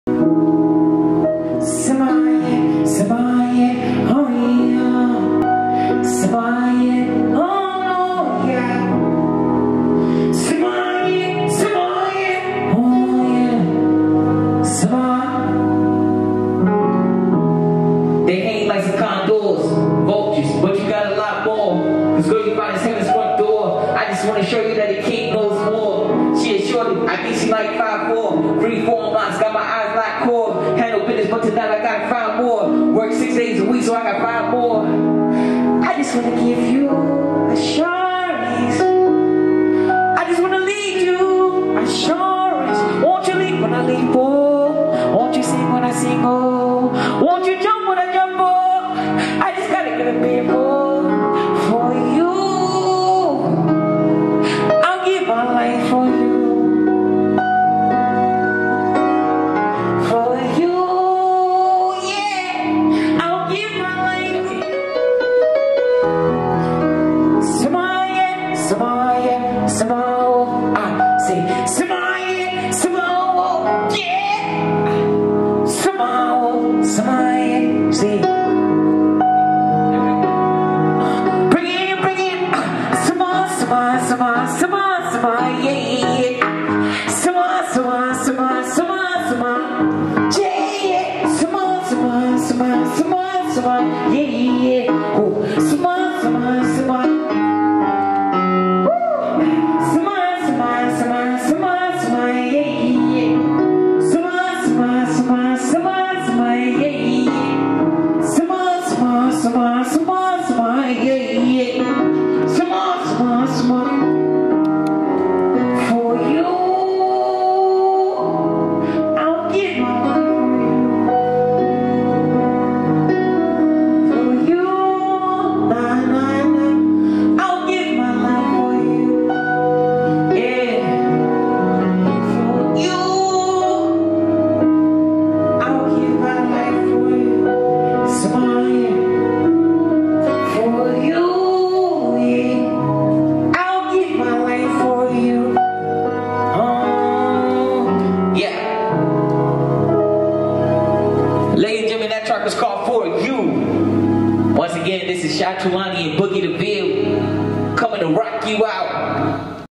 Chai Tulani Live virtual show
A few clips Shot by @trapula.ashe  for a set we did for @softcagefilms

Show us curated by @locklady1 @humblequestreunioncommittee @haveaheartgiveaheart

Shout out to @schoolofrockoakpark for allowing us to use their space